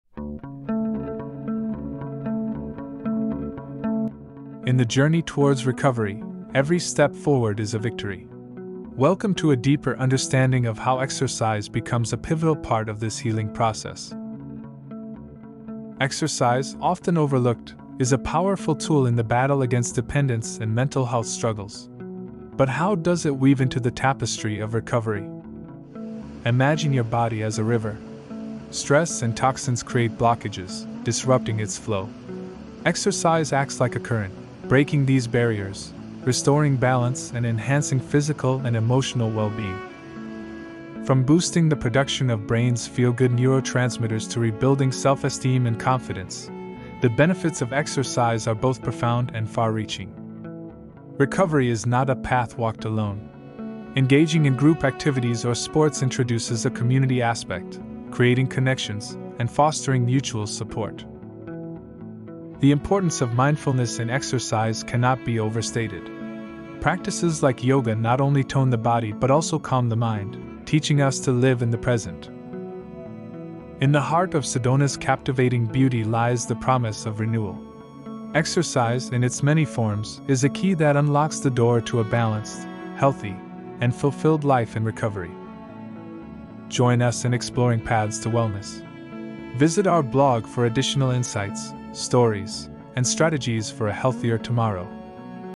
The Importance of Exercise for Mental Health and Recovery
Discover the critical role exercise plays in the recovery process with our latest video, 'Unlocking Recovery: The Transformative Power of Exercise'. Journey with us through the stunning landscapes of Sedona, Arizona, as we explore how physical activity not only revitalizes the body but also heals the mind. From enhancing mental health to fostering community connections, learn why exercise is a vital component of recovery and how it can lead to a more balanced, fulfilling life.